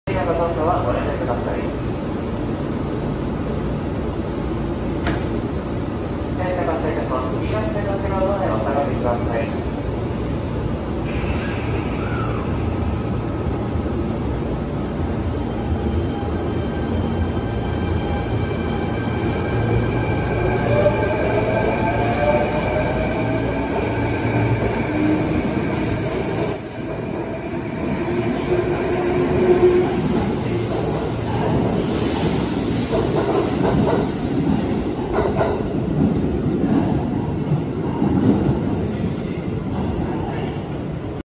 【近鉄5820系】DH25（5325F）大和西大寺行き区間準急 発車
撮影：大阪難波駅（2021/07/03）